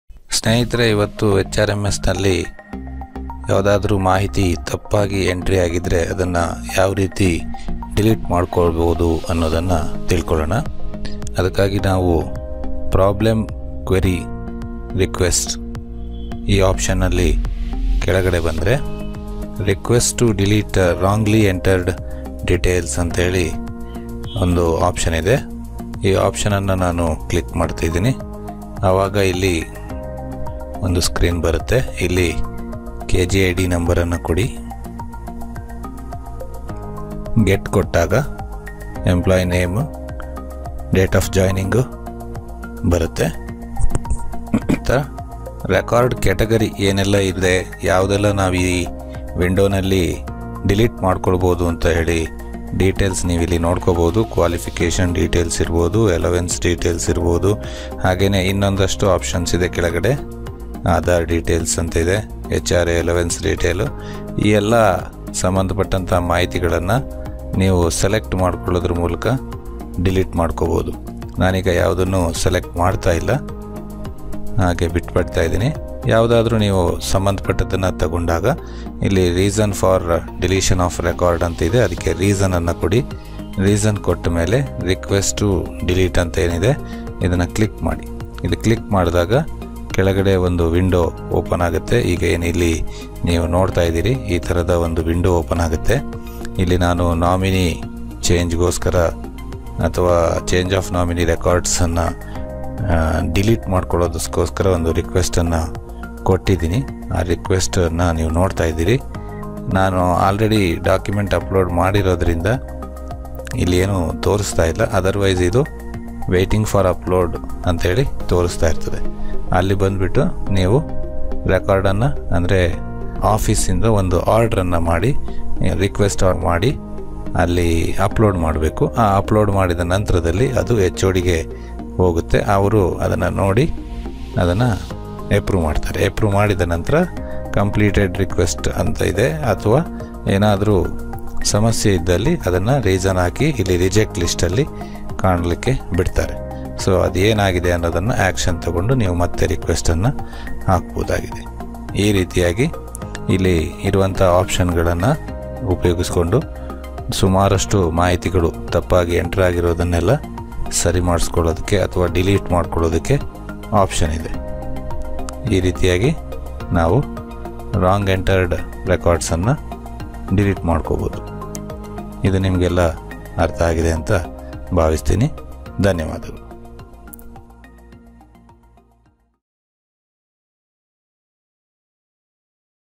How to delete wrong details from hrms.
In case of wrong entries entered in hrms you can delete them with this method.